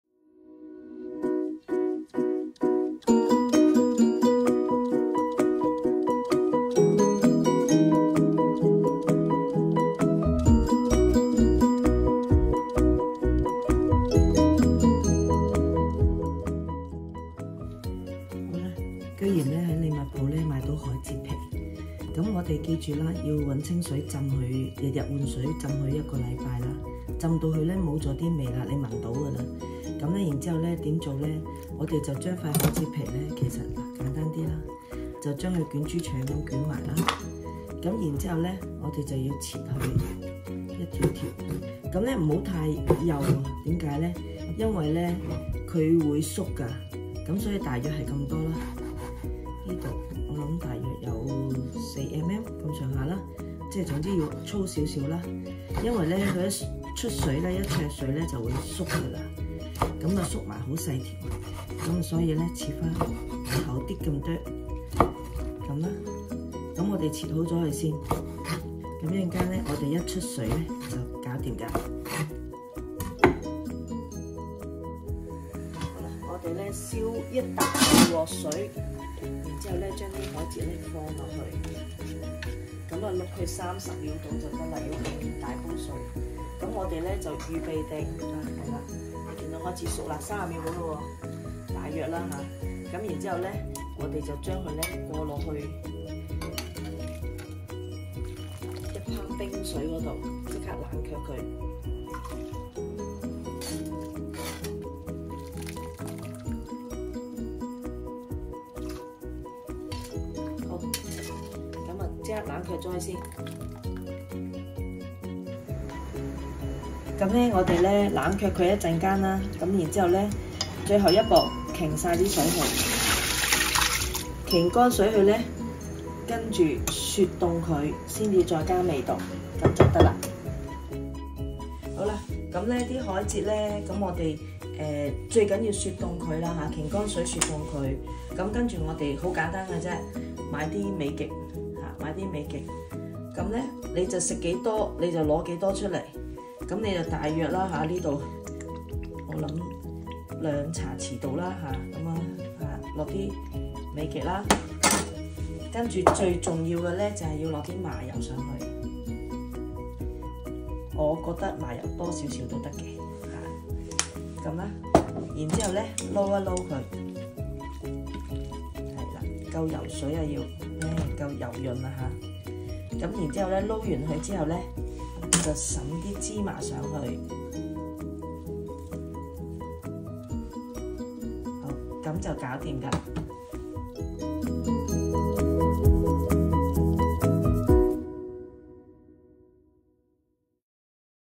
麻油海蜇
沒想到在英國中超（Liverpool )居然找到鹽水海蜇皮，當然不會放過它，因為我自小就很喜歡吃「橡筋」。媽媽教我怎樣處理和調味，不麻煩但花時間，希望大家有機會試試看。

材料：
鹽水海蜇皮一包（約1kg)
美極鮮醬油或生抽
麻油

海蜇皮處理
1.去掉鹽水、沖沖水
2.用一盆清水浸泡
3.每日更換清水
4.大約5-7天（直到沒有咸水味道）
5.將海蜇皮摺起，切成粗條
6.燒熱一大鍋水
7.放入海蜇條
8.浸泡約30秒撈起
9.即浸泡冰水降溫
10.幾分鐘後，隔去所有冷卻水份
11.放入雪櫃，最少一天

調味：
*記得吃之前才調味道
*吃多少海蜇，就調多少味道

大約250克海蜇，用2-3茶匙美極鮮醬油、3-4茶匙麻油，撈勻後，灑上芝麻即成。